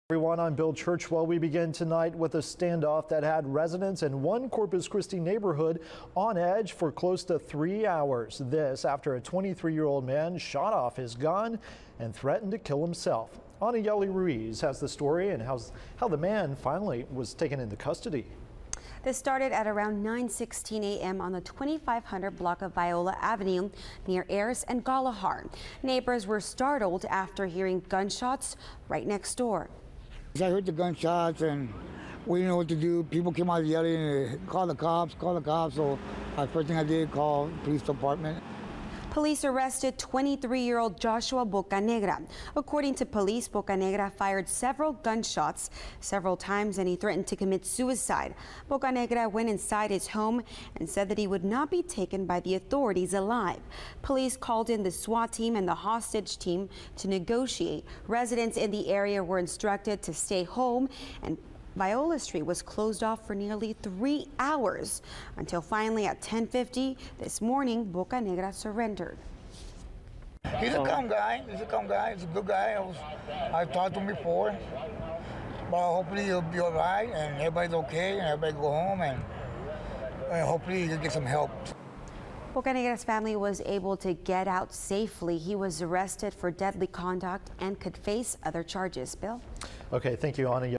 Corpus Christi TX~Police & SWAT Team Surround Home
Police got a call on Ayers & Viola in reference to a man who discharged a firearm and then locked himself in his home. The SWATT team was called out to the scene. The man has surrendered safely.